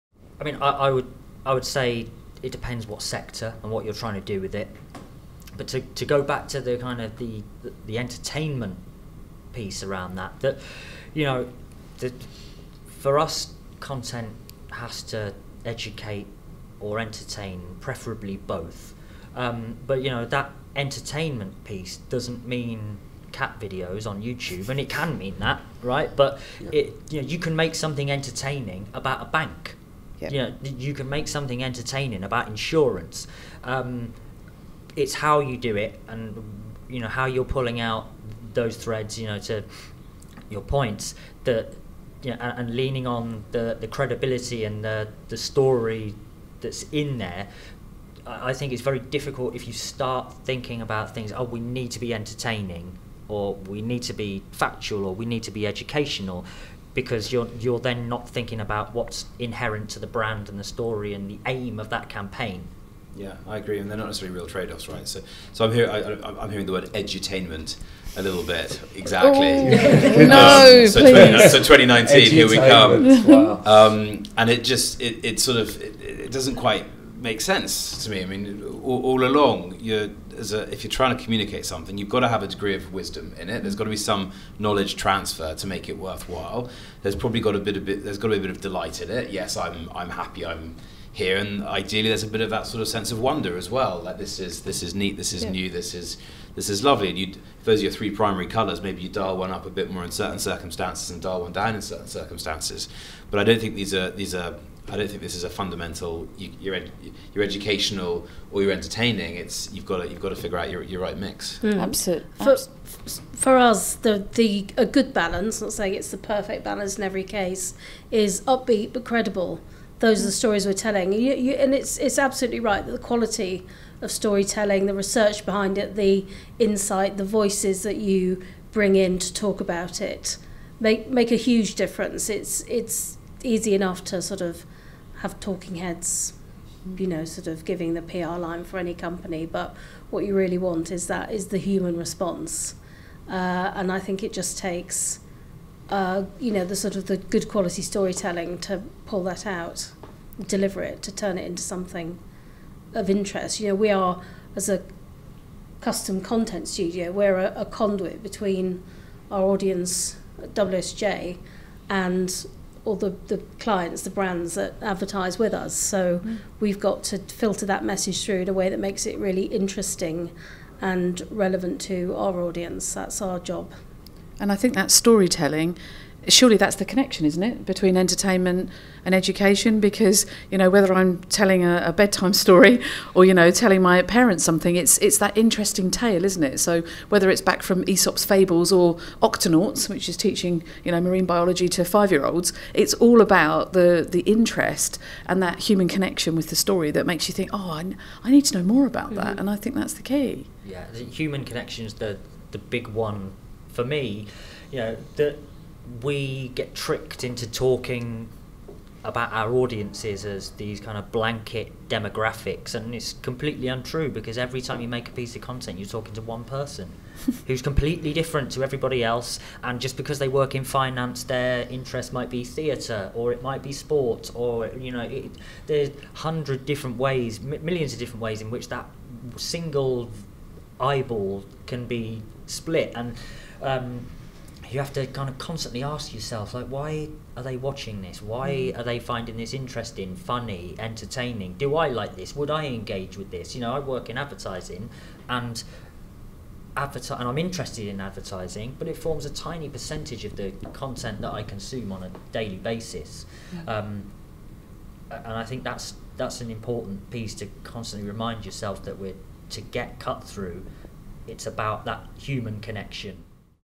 What does thought leadership look like and what does that mean for clients in 2019?
Roundtable at NewsUK:
Festival of Media CEO Jeremy King sat with industry experts to discuss what thought leadership looks like and what it means for clients in 2019?

Jackie Hunter, Executive Editor, EMEA & APAC, Dow Jones
Jim Piercy, Creative Director, EMEA & APAC, Dow Jones
Samantha Fay, Senior VP, Global Brand Strategy, Guinness Book of Records
Harry Wake, President, EMEA, Group SJR